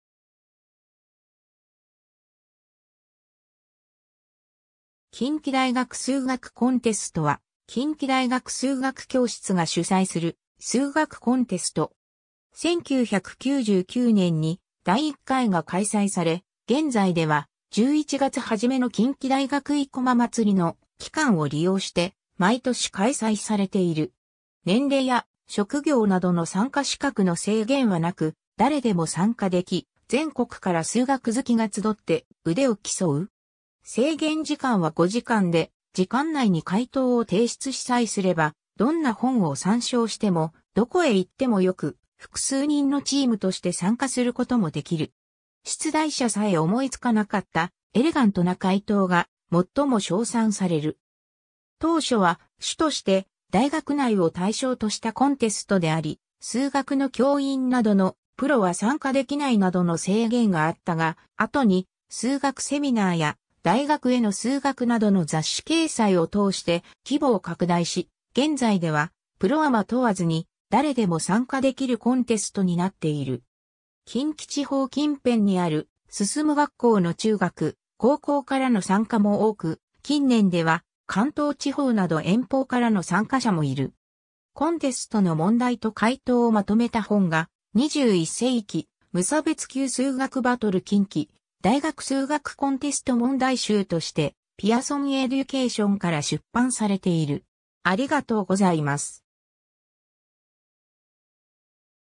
近畿大学数学コンテスト
近畿大学数学コンテスト（きんきだいがくすうがくコンテスト、英語：Kinki University Mathematics Contest）は、近畿大学数学教室が主催する数学コンテスト。
 1999年に第一回が開催され、現在では、11月はじめの近畿大学生駒祭の期間を利用して、毎年開催されている。
 年齢や職業などの参加資格の制限は無く、誰でも参加でき、全国から数学好きが集って腕を競う。
制限時間は5時間で、時間内に解答を提出しさえすれば、どんな本を参照しても、どこへ行っても良く、複数人のチームとして参加することもできる。
出題者さえ思いつかなかった“エレガント”な解答が、最も賞賛される。
 当初は、主として大学内を対象としたコンテストであり、数学の教員などの「プロ」は参加できないなどの制限があったが、後に『数学セミナー』や『大学への数学』などの雑誌掲載を通して規模を拡大し、現在ではプロ・アマ問わずに誰でも参加できるコンテストになっている。
近畿地方近辺にある進学校の中学・高校からの参加も多く、近年では関東地方など遠方からの参加者もいる。
 コンテストの問題と解答をまとめた本が、『21世紀無差別級数学バトル　近畿大学数学コンテスト問題集』としてピアソン・エデュケーションから出版されている。